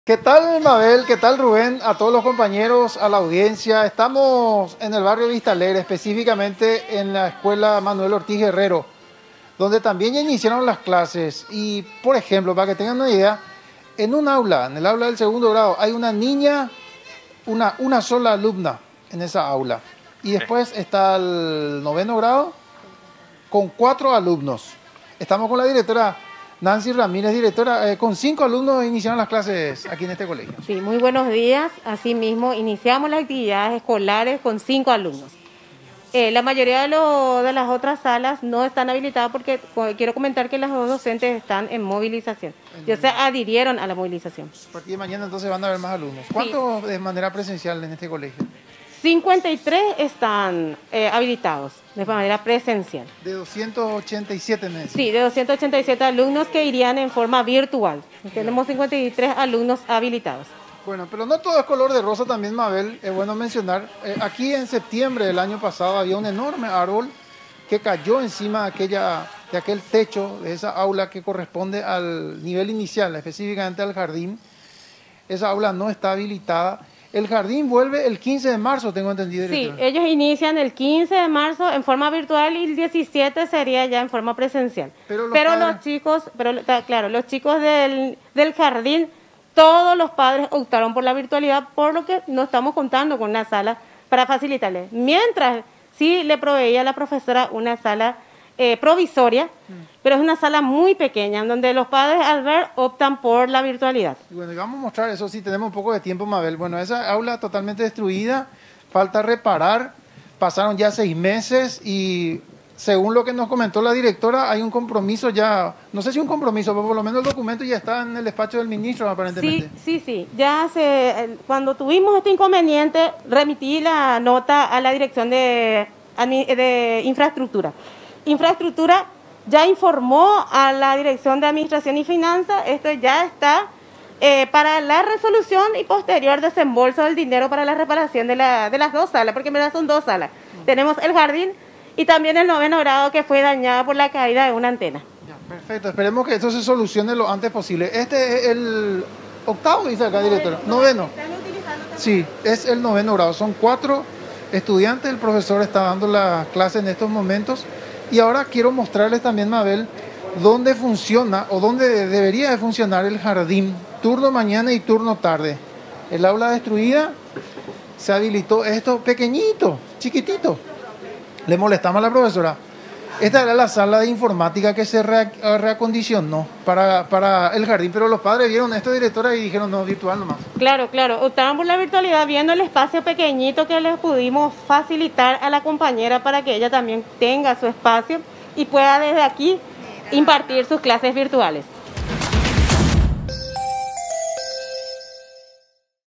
Arrancaron las clases presenciales y virtuales en el colegio Manuel Ortiz Guerrero
A pesar de las malas condiciones de parte de la infraestructura del colegio Manuel Ortiz Guerrero, hoy arrancaron las clases presenciales con la presencia de un puñado de estudiantes.